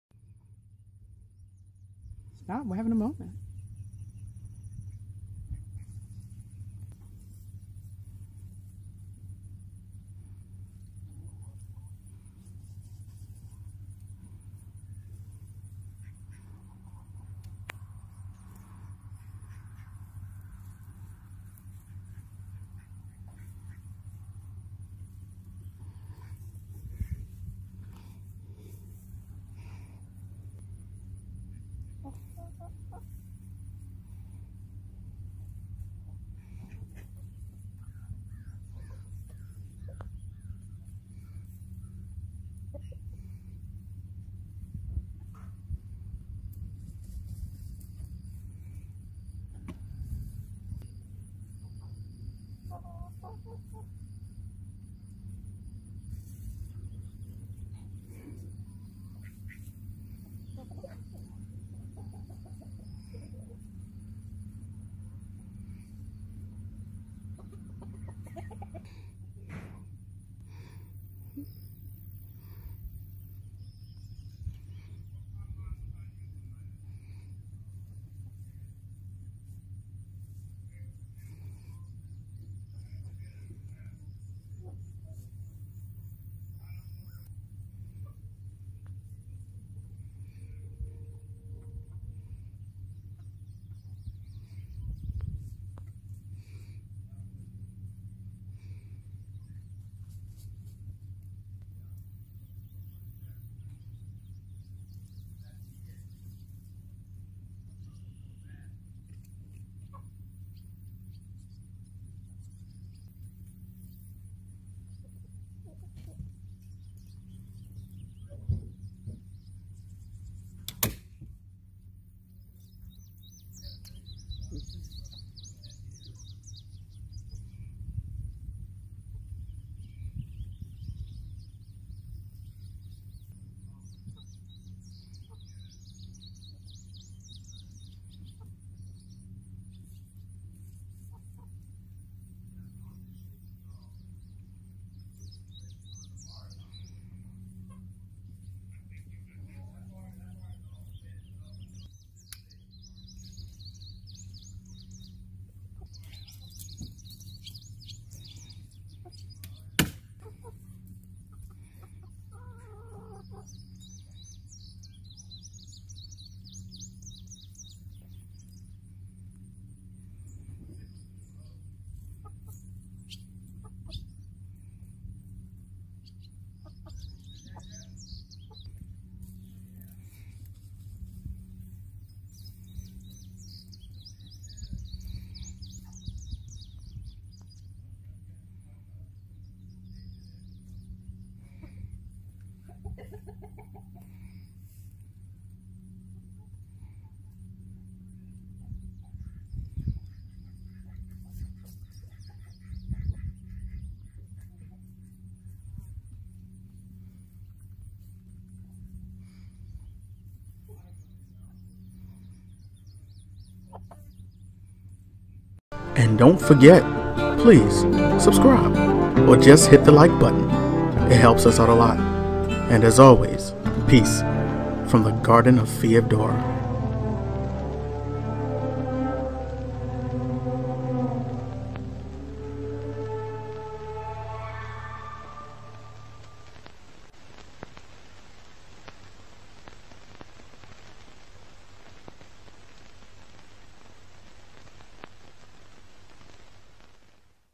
Petting a Dragonfly in THE GARDEN OF FEODORA
This Dragonfly let us get close enough to pet, touch and play with it.